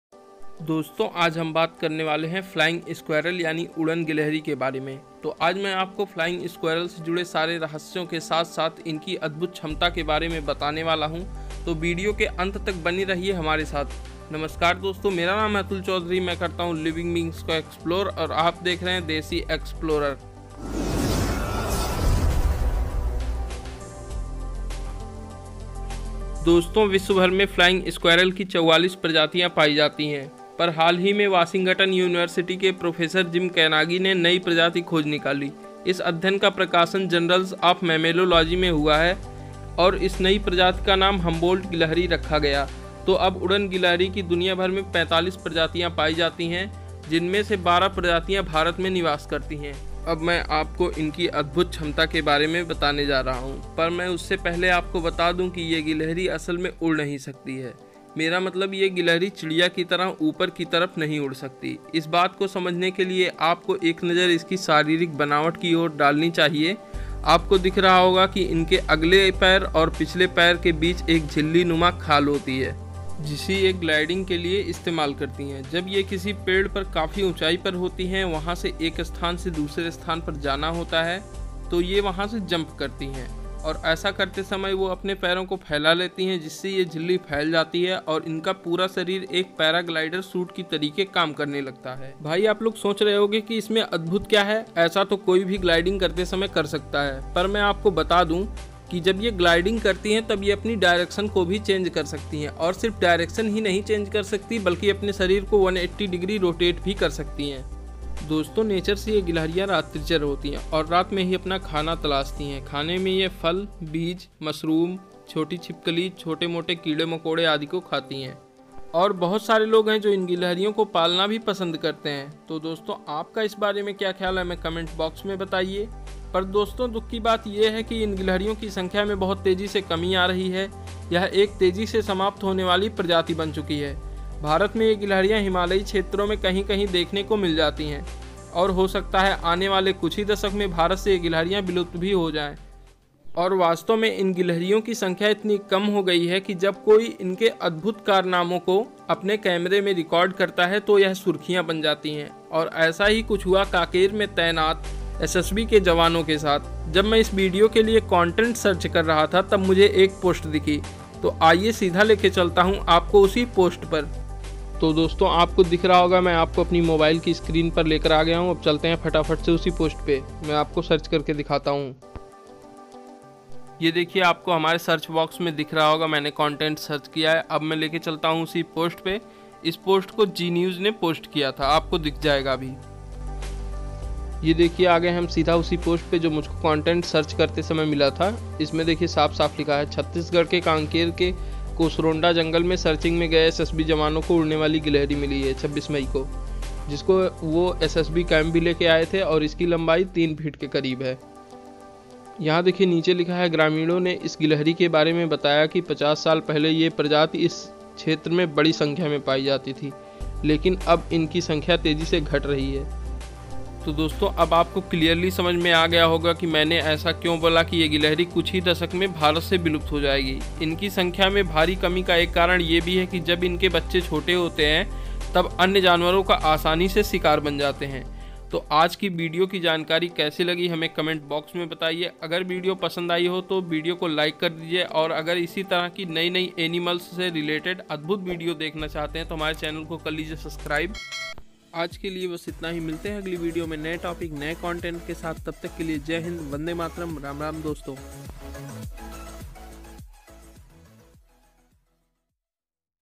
क्या भारत से विलुप्त हो जाएगी उड़न गिलहरी | Flying squirrels | by DESI explorer
About me-
My self atul chaudhary and welcome to the my channel DESIexplorer

about video-
dosto aaj hum baat karne waale hai udan gilahri ke baare me jise log flying squirrel bhi kahte hai to aaj mai aapko inki adbhud chamta ke baare me batane wala hu dosto vishwa bhar me inki 44 prajatiyan paayi jaati hai jinme se 12 prajatiyan to bharate hi paayi jaati hai ye to ho gayo udan gilahri ki prajatiyan.
ab baat karte hai inki adbhud chamta ke baare me -
yah ek unique animal hai inka naam unique animals ki list me jaroor dalna chahiye is gilahri khaas baat ye hai ki ye uad to nhi sakti balki ye hawa me glaiding karti hai .

in english-

Flying squirrels are a tribe of 50 species of squirrels in the family Sciuridae. They are not capable of flight in the same way as birds or bats but are able to glide from one tree to another with the aid of a patagium, a furry, parachute-like membrane that stretches from wrist to ankle. Their long tail provides stability in flight. Anatomically they are very similar to other squirrels but have a number of adaptations to suit their life style; their limb bones are longer and their hand, foot bones and distal vertebrae are shorter. Flying squirrels are able to steer and exert control over their glide path with their limbs and tail.

pFlying squirrels are not capable of flight like birds or bats; instead, they glide between trees.